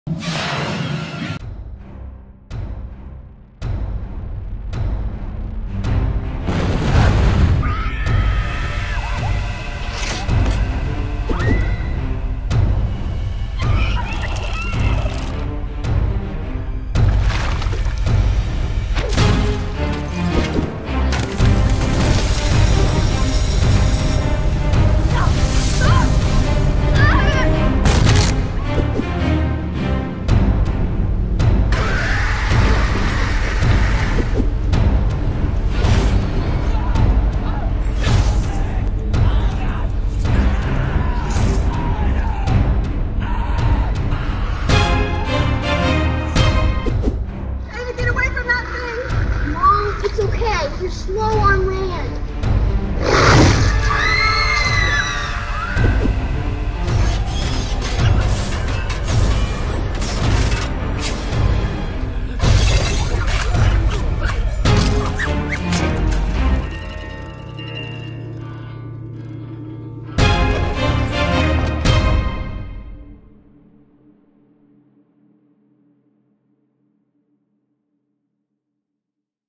Best Horror Movie Deaths
True horror fans know: Some deaths are better than others. Here are some of the finest movie kills.